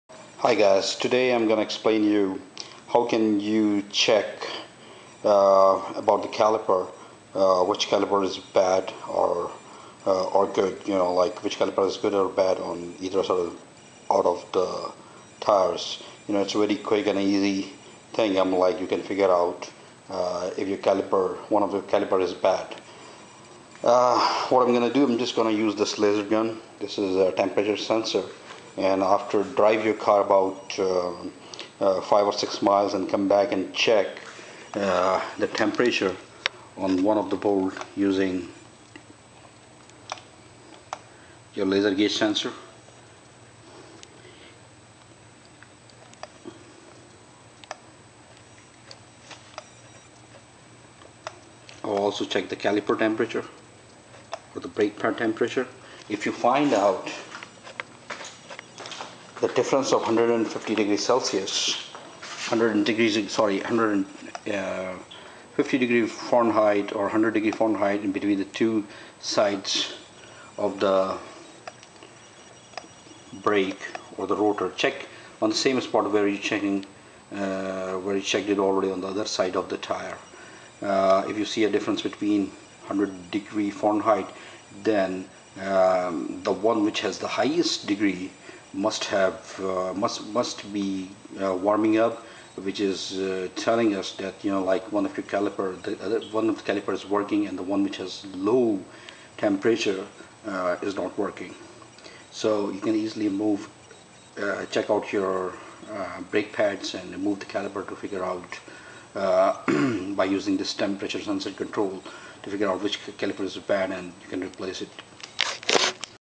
Trick to check Bad Caliper how to check quickly
If one of your calipers gone bad, Use temperature sensor to find out the difference b/w them. The side which has bad caliper will be sensing lower